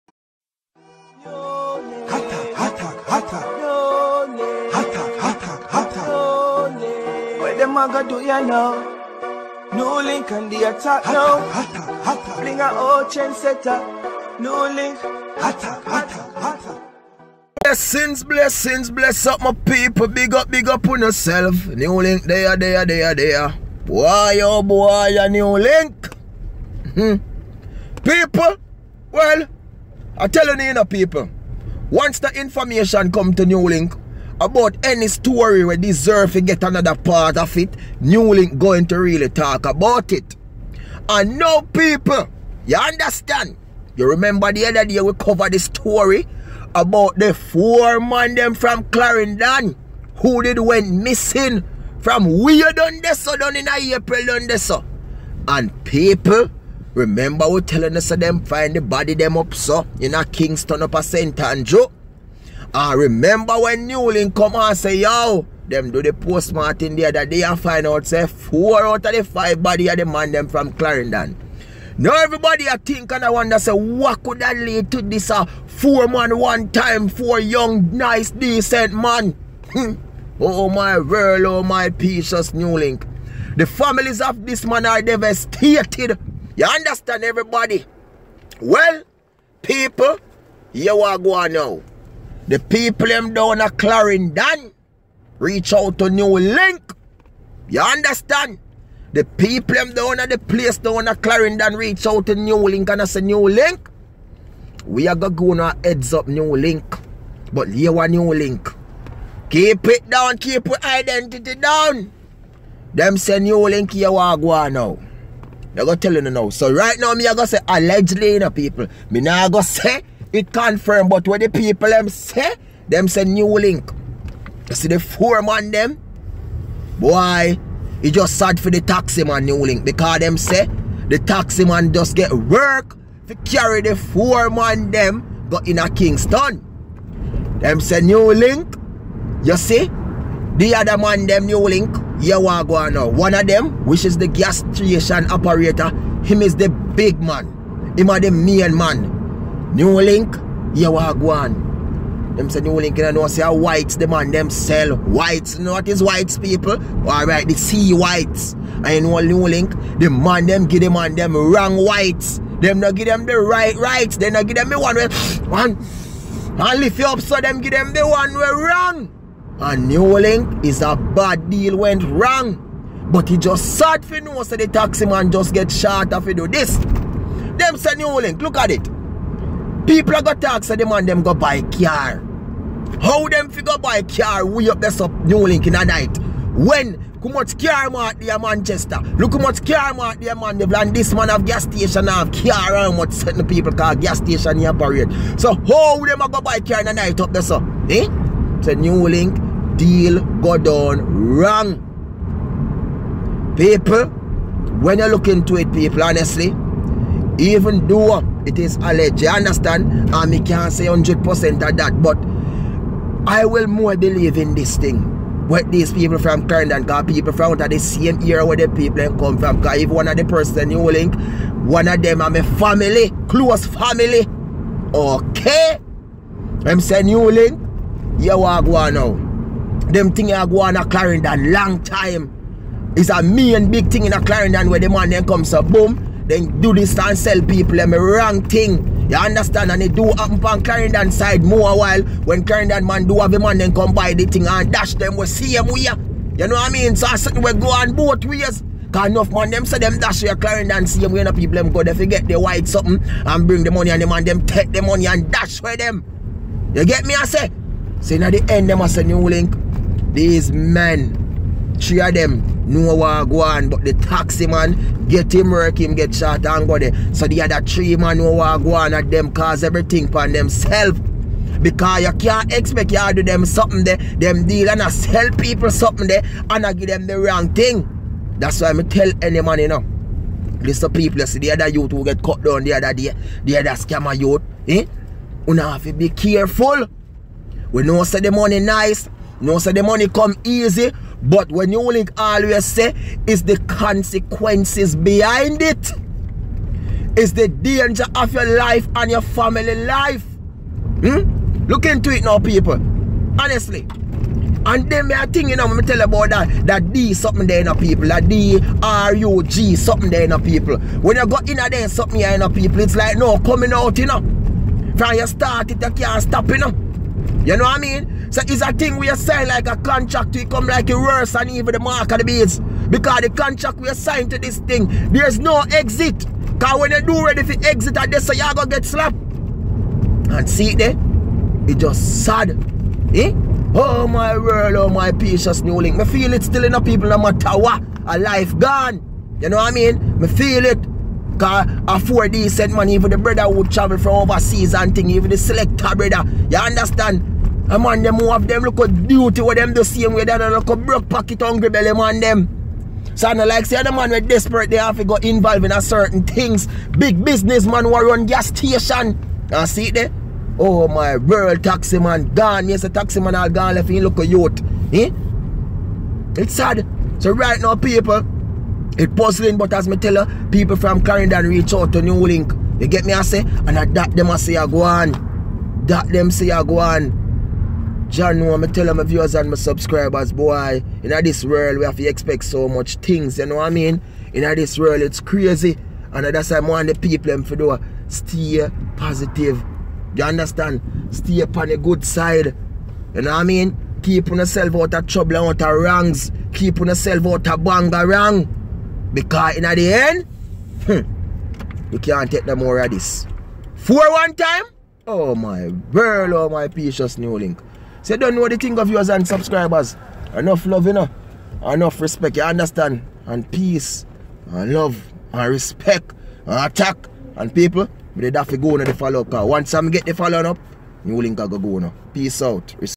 BREAKING FULL R3AS0N 4 CLAREND0N T0P M3N WAS KI((3D AND BUR!3D IN KINGSTON (ALLEG3DLY)
GREETINGS MY VIEWERS AND SUBSCRIBERS TODAY I SHARE WITH YOU ANOTHER STORY...THIS TIME IT'S AN UPDATE ON THE RECENT STORY I COVERED ABOUT THE 4 MEN WHO WENT MISSING FROM CLARENDON JAMAICA...PLEASE WATCH AND YOUJOY YOURSELF AS I SHARE WITH YOU...